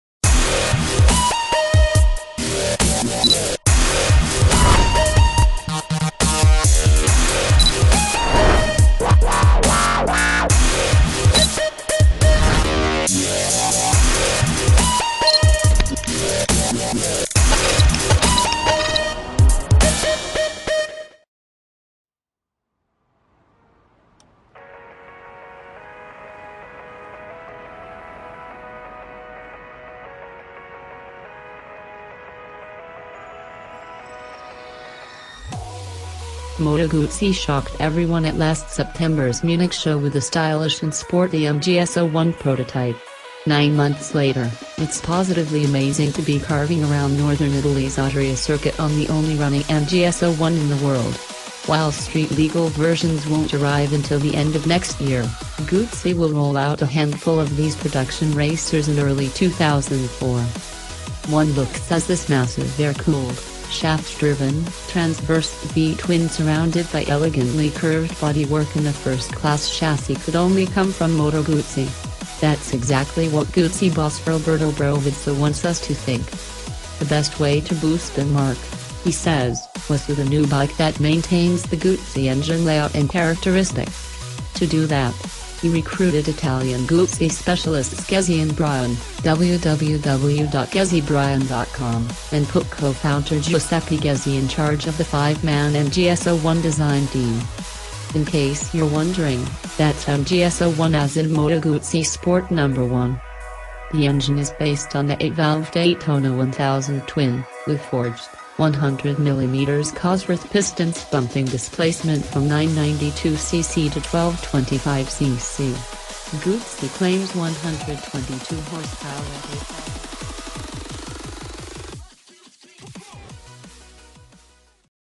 WOW!! Best Features Moto Guzzi MGS01 But Users Don't Know About This
WOW!! Best Features Moto Guzzi MGS01 But Users Don't Know About This - Moto Guzzi shocked everyone at last September's Munich Show with the stylish and sporty MGS-01 prototype. Nine months later, it's positively amazing to be carving around Northern Italy's Adria circuit on the only running MGS-01 in the world. While street-legal versions won't arrive until the end of next year, Guzzi will roll out a handful of these production racers in early 2004.

One look says this massive air-cooled, shaft-driven, transverse V-twin--surrounded by elegantly curved bodywork and a first-class chassis--could only come from Moto Guzzi. That's exactly what Guzzi boss Roberto Brovazzo wants us to think. "The best way to boost the marque," he says, "was with a new bike that maintains the Guzzi engine layout and characteristics." To do that, he recruited Italian Guzzi specialists Ghezzi & Brian and put co-founder Giuseppe Ghezzi in charge of the five-man MGS-01 design team. In case you're wondering, that's MGS-01 as in "Moto Guzzi Sport number one."

$34,950